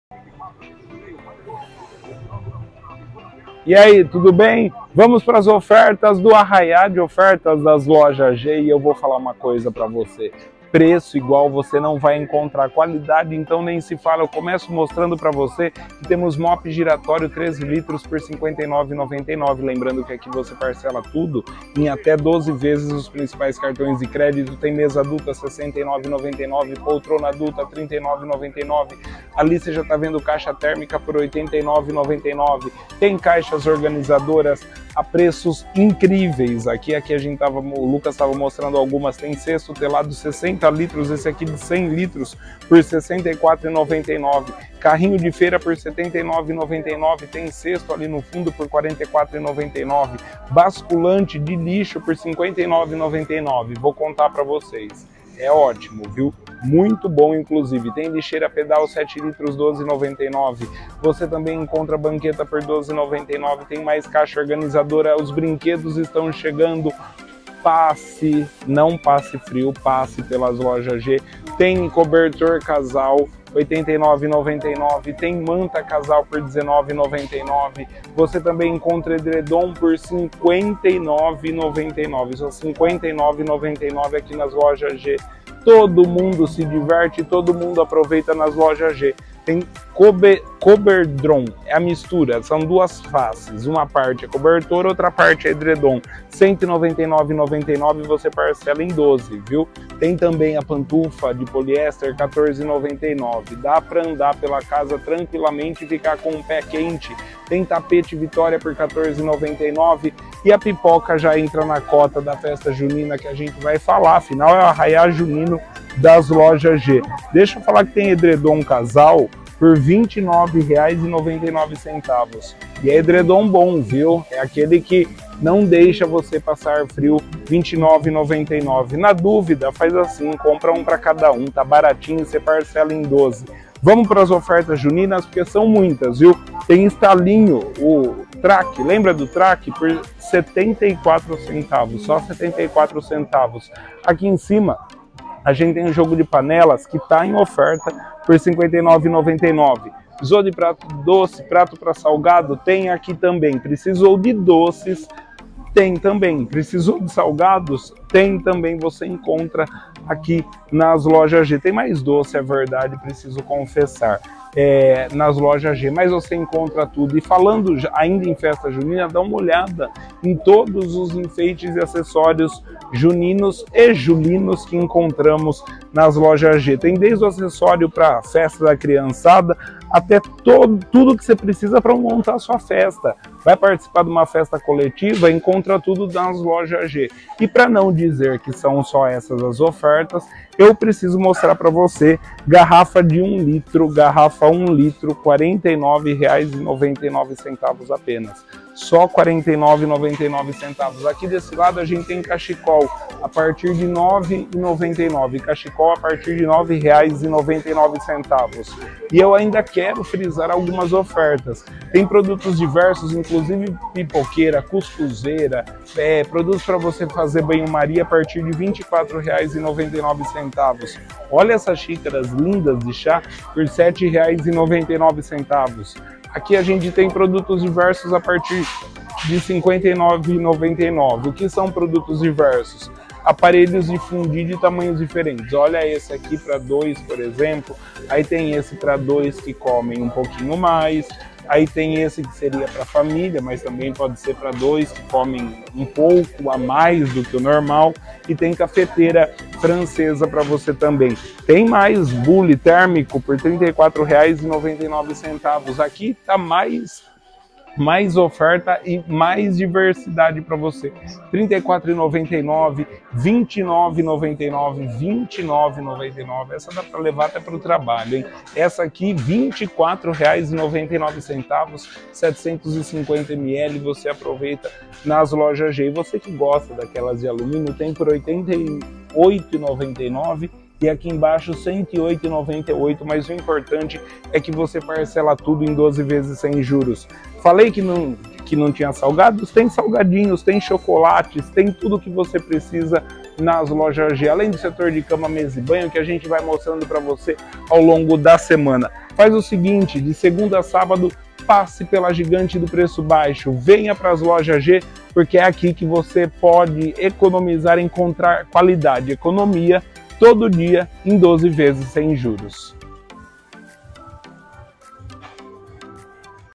Arraia de ofertas e tudo em até 12x sem juros só nas Lojas G
Venha para a Gigante do preço baixo em Jaú

Ofertas arrasadoras e preço baixo todo dia, você só encontra aqui

Na GIGANTE do seu lar aqui na rua Edgar Ferraz, você compra de segunda a sábado.
Economia de verdade é na gigante da região, que ainda divide tudo em até 12x nos principais cartões de crédito.
Cama, mesa, banho, brinquedos, presentes, material escolar, cutelaria, utensílios de cozinha, panelas, cortinas... aqui tem tudo que você precisa.

LOJAS G - A GIGANTE DO SEU LAR
Rua Edgar Ferraz, 262 - Jaú